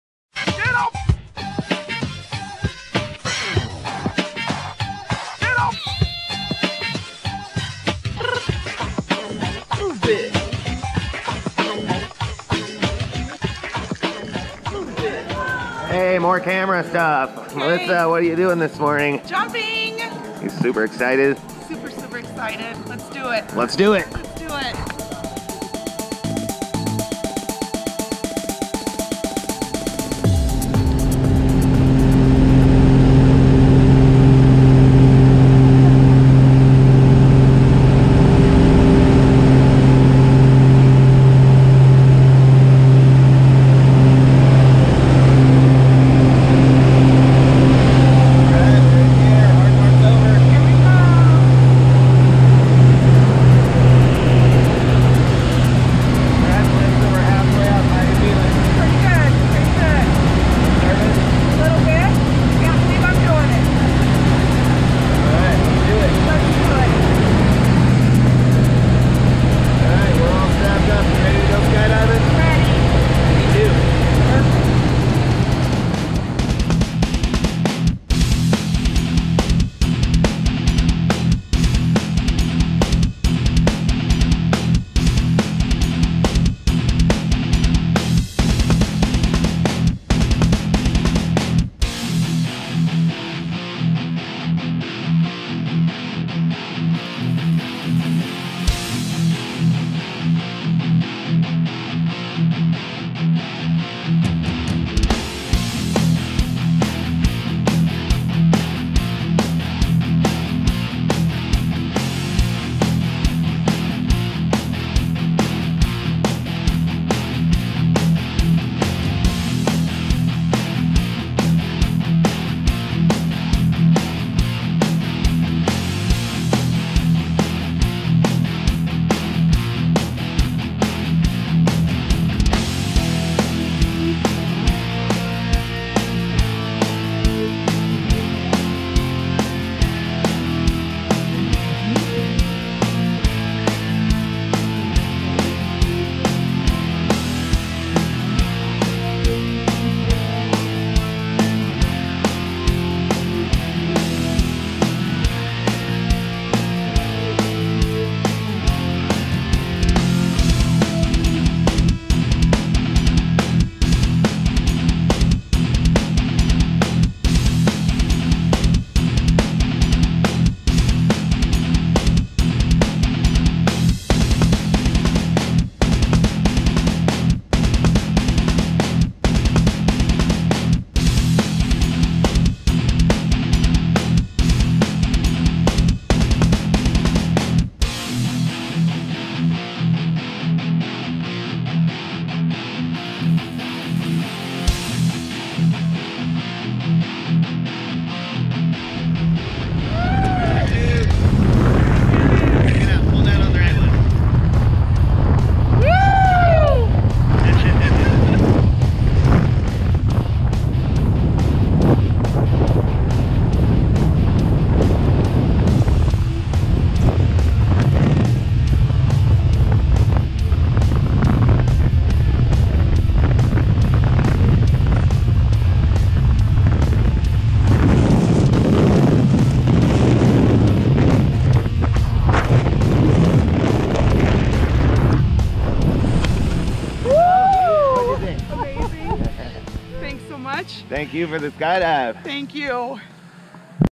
Melissa Kennedy's Tandem skydive!
Melissa Kennedy's tandem skydive on Saturday, September 22, 2018 with Eric Skroch.